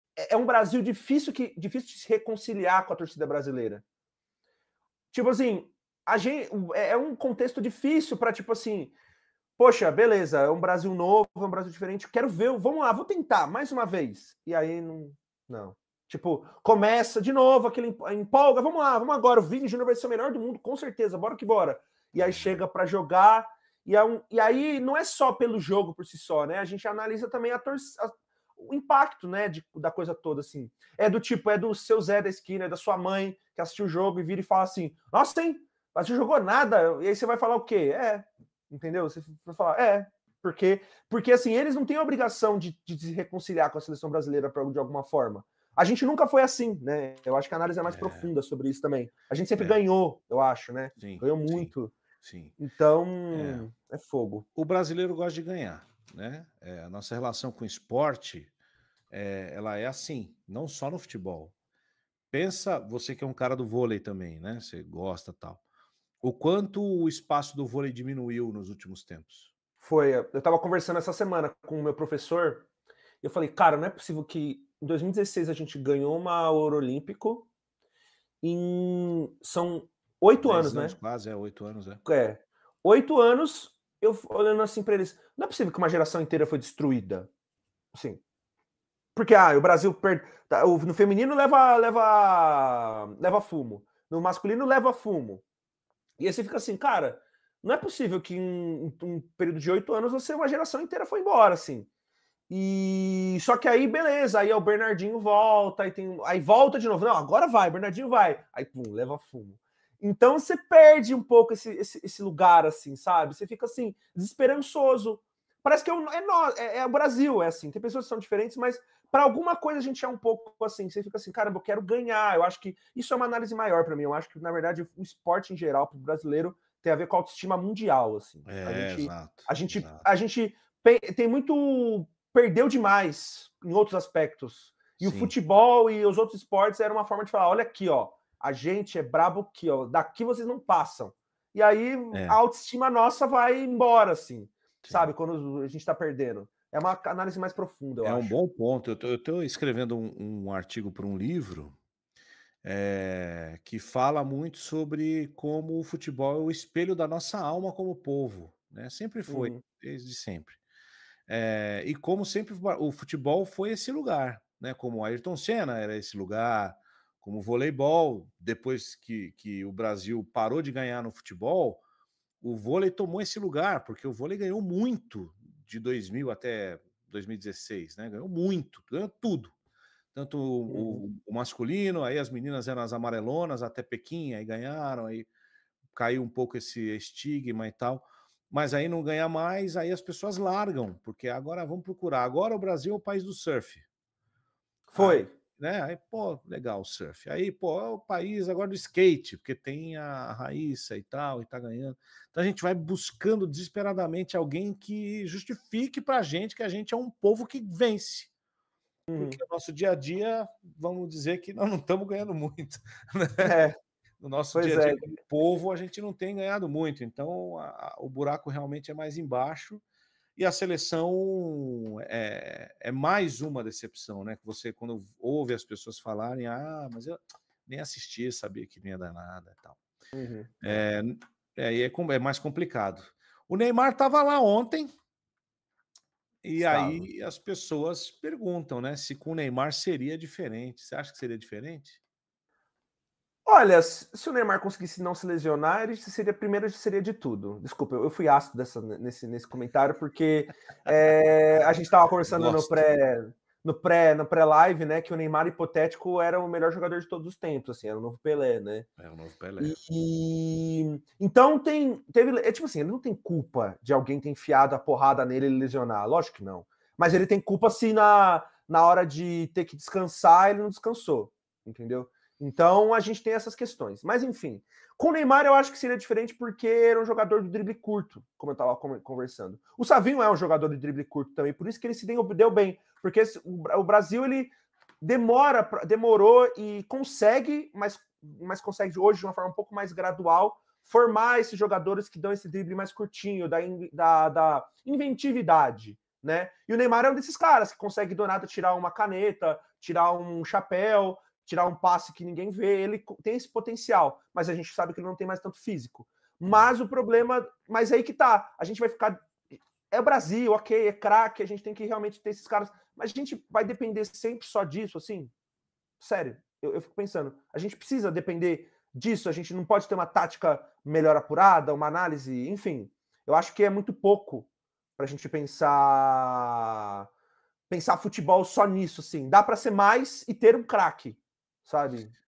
Brasileiros e Esportes: O futebol é o espelho da nossa alma como povo | Cortes do Caçapa
Nesse vídeo, eu e Gabriel de Nani falamos sobre a relação dos brasileiros com os esportes, como se perdeu a fé na Seleção brasileira, Ayrton Senna e até sobre o período de glória da Seleção de vôlei.
Qual é a sua opinião? Deixe aqui nos comentários!